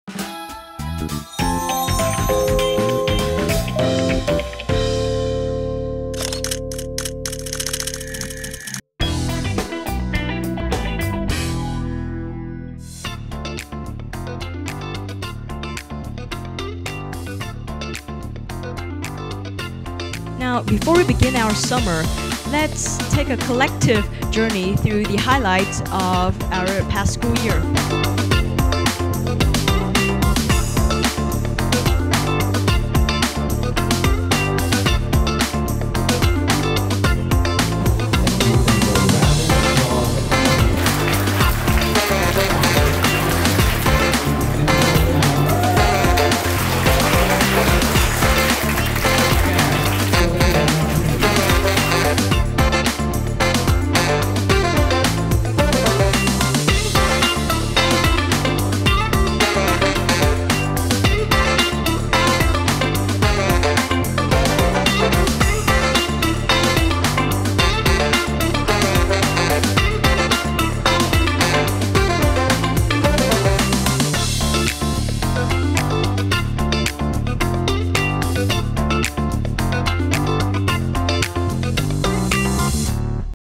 Nam Uc Scotch AGS Saigon Center | Best Moments of the 2023-2024 Year-End Ceremony
✨ Best Moments of the Year-End Ceremony of the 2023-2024 School Year - Nam Uc Scotch AGS Saigon Center

🥳 The Year-End Ceremony at the Saigon Center campus was filled with joy, happiness, and pride. On the last day of school, our Years 6-11 Scotchies, esteemed teachers, and dedicated staff came together to reflect on a journey of effort and growth, express heartfelt gratitude, and share the beautiful memories we created throughout this incredible year.

🎉 Once again, Nam Uc Scotch AGS congratulates all our outstanding students and inspiring teachers at the Saigon Center campus on completing the remarkable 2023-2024 academic year. Let us embrace the summer break with excitement and anticipation for even more impressive achievements in the future!
✨ Cùng nhìn lại những khoảnh khắc đáng nhớ trong buổi Lễ Bế giảng năm học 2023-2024 tại cơ sở Trung tâm Sài Gòn

🥳 Lễ Bế giảng năm học tại Nam Úc Scotch AGS - cơ sở trung tâm Sài Gòn vừa qua đã diễn ra trong niềm vui và sự tự hào. Trước khi tạm chia tay để nghỉ hè, các bạn Scotchies Khối 6-11 và thầy cô đã có dịp cùng nhau chúc mừng, bày tỏ lòng biết ơn chân thành cho những nỗ lực và thành tựu đạt được, cũng như ôn lại các kỷ niệm đẹp của năm học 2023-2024.

🎉 Một lần nữa, Nam Úc Scotch AGS xin gửi lời chúc mừng tập thể cơ sở Trung tâm Sài Gòn đã có một năm học đáng nhớ. Chúc mọi người nghỉ hè thật vui và gặt hái được nhiều thành quả ấn tượng hơn nữa trong năm học sắp tới nhé!